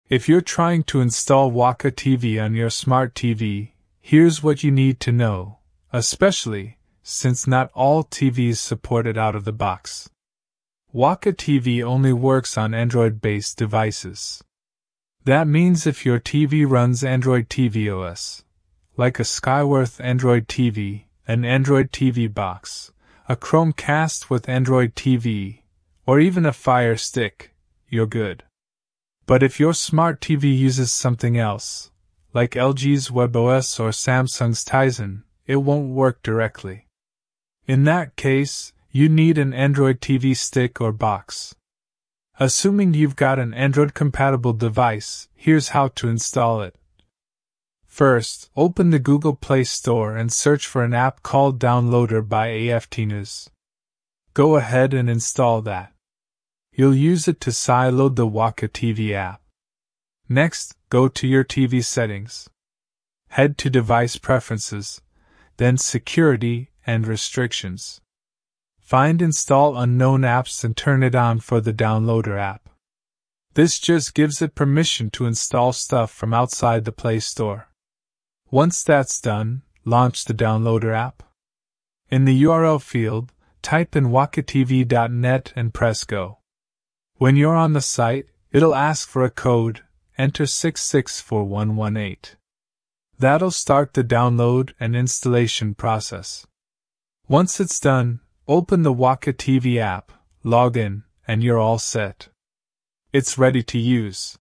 How To Install Waka TV On Smart TV In 2026?
In this comprehensive tutorial, we will guide you through the step-by-step process of installing Waka TV on your Smart TV.

Thank you for your support!

 Explore the Best Deals
► If you are purchasing from Amazon, you can use this